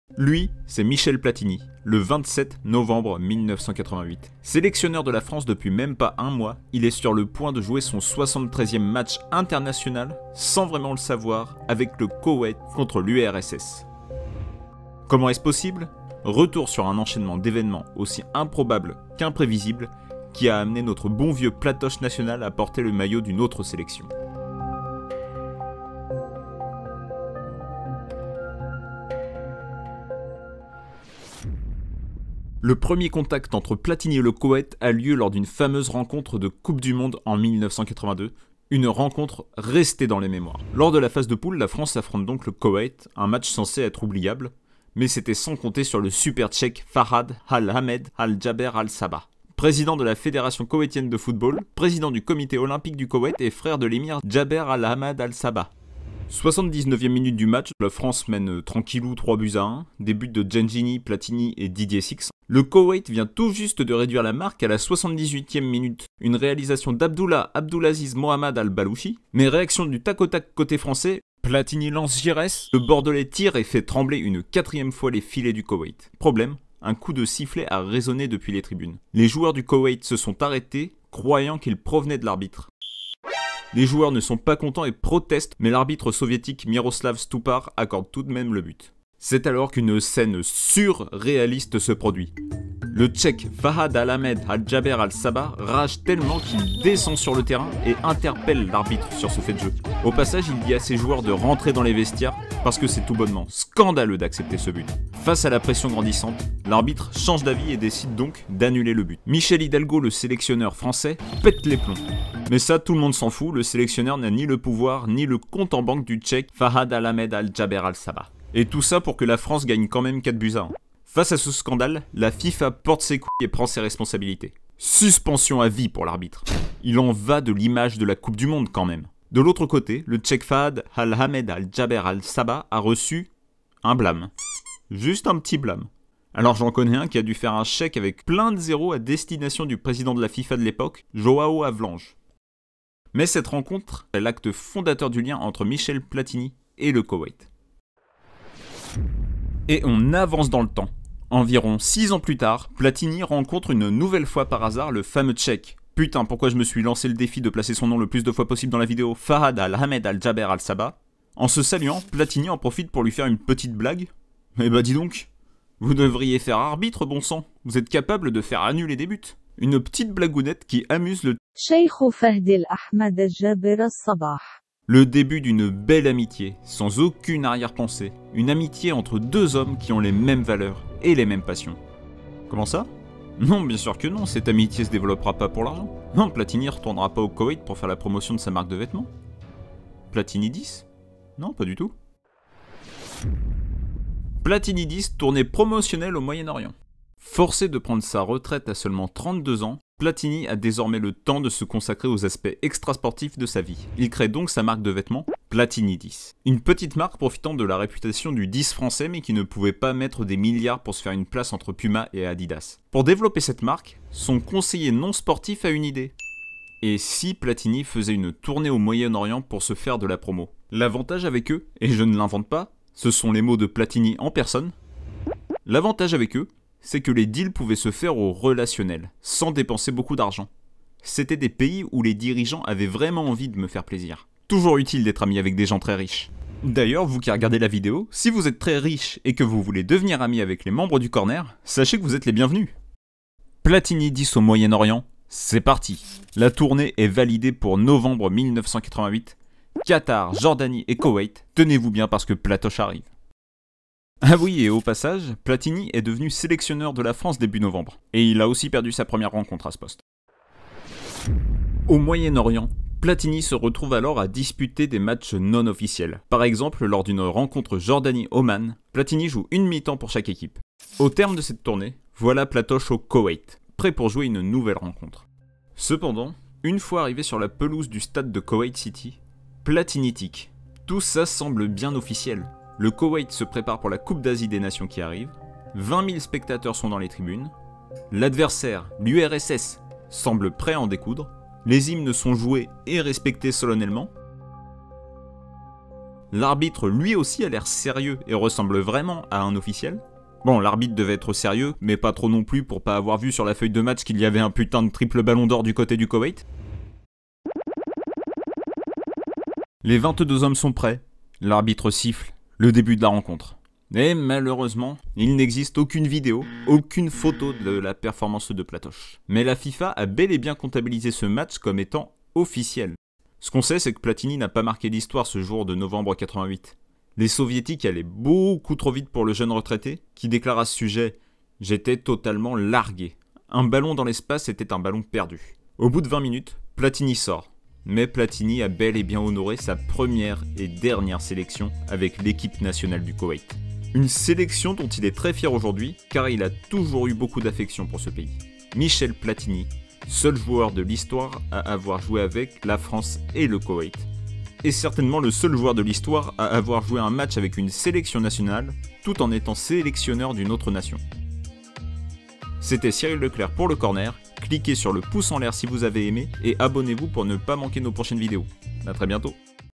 Michel PLATINI a une sélection avec le KOWEIT ?!
Michel Platini compte une sélection avec le Koweït, mais comment est-ce possible ? Retour sur un enchaînement d'événements aussi imprévisible qu'improbable.

Et, contrairement à ce que je vous ai dit dans la vidéo, des images de cette rencontre existent

Michel Platini, légende de l'équipe de France, de l'AS Saint-Etienne et de la Juventus est obligé de prendre sa retraite à seulement 32 ans. Un an après, lors d'une tournée promotionnelle pour sa marque de vêtement "Platini 10", le sélectionneur de l'équipe de France se retrouve à jouer un match officiel avec la sélection nationale du Koweït. Le tout étant parti d'un épisode sur-réaliste, quand l'Emir Fahad Al-Ahmed Al-Jaber Al-Sabah est descendu sur le terrain, lors de la Coupe du monde 1982, pour demander à l'arbitre soviétique, Myroslav Stupar, d'annuler le but inscrit par Alain Giresse. Pourquoi ? A cause d'un coup de sifflet provenant des tribunes, qui auraient incité les joueurs du Koweït à arrêter de jouer...

Sources :
- Calendrier SoFoot, 27 novembre.
- Pelé, Barthez, Roberto Carlos, Platini… Ces joueurs revenus sur leur retraite sportive, France Football
- Dix petites histoires sur Platini, le potentiel futur big boss du foot mondial, Capital
- Mythes Téléfoot : Le cheikh du Koweït annule un but de la France à la Coupe du Monde
- JO 2016, le Brésil veut oublier Joao Havelange, le ‘papi de la corruption’

Pour en voir plus sur Michel Platini, l'un des trois meilleurs joueurs de l'histoire de l'équipe de France :